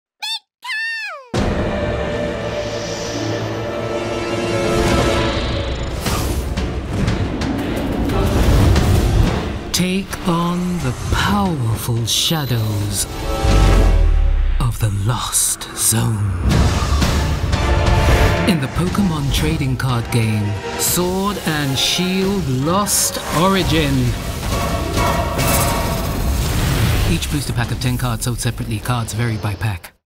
Pokémon TCG: Sword & Shield—Lost Origin | Available Now! 🌑🌌
New Radiant Pokémon, more Pokémon VSTAR, and the return of the Lost Zone in the latest Pokémon TCG expansion, Sword & Shield—Lost Origin, available now! 🌑🌌